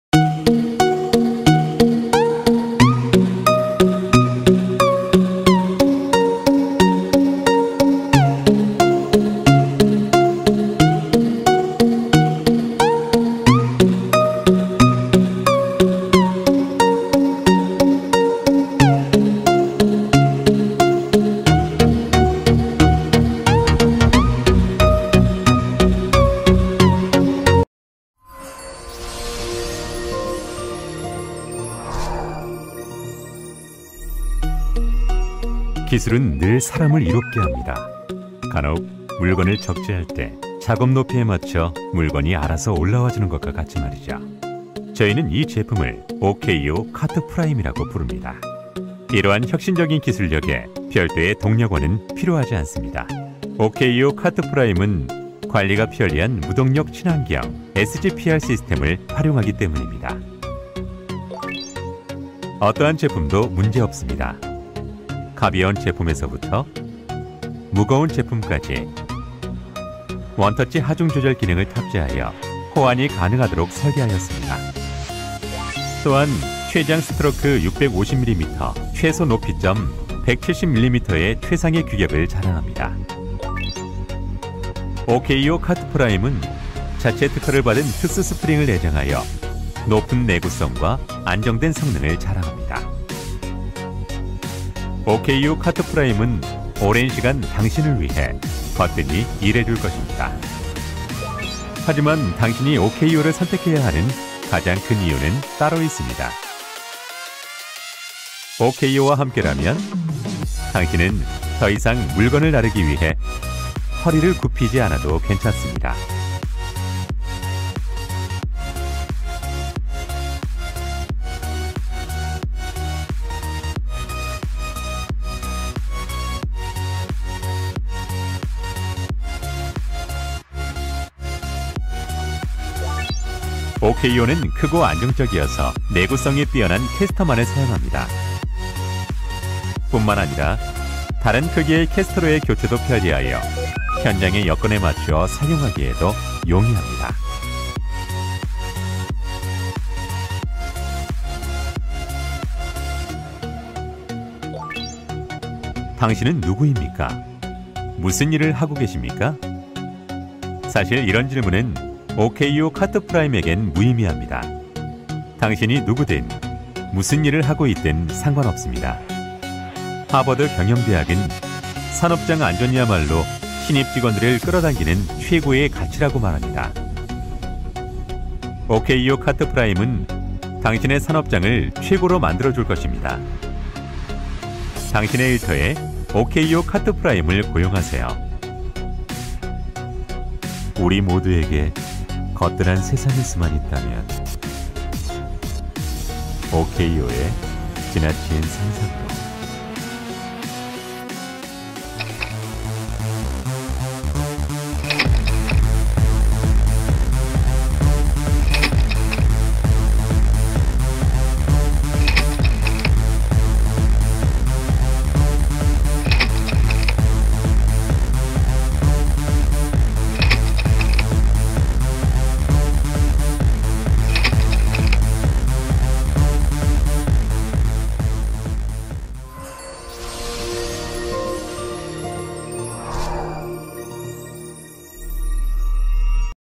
신기한 허리보호 높낮이조절 카트 [오케이오]
물건의 무게에 따라서 상판이 근로자의 허리높이까지 유지되어
물건을 적재, 하역시에 근로자가 허리를 굽히지 않아도 되는
인간공학적 카트입니다.

U.S. and lnternational Sales Office
 Address : 3820 Del Amo Blvd.

EU Sales Office
 Address : Graf Adolf Str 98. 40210 Duesseldorf Germany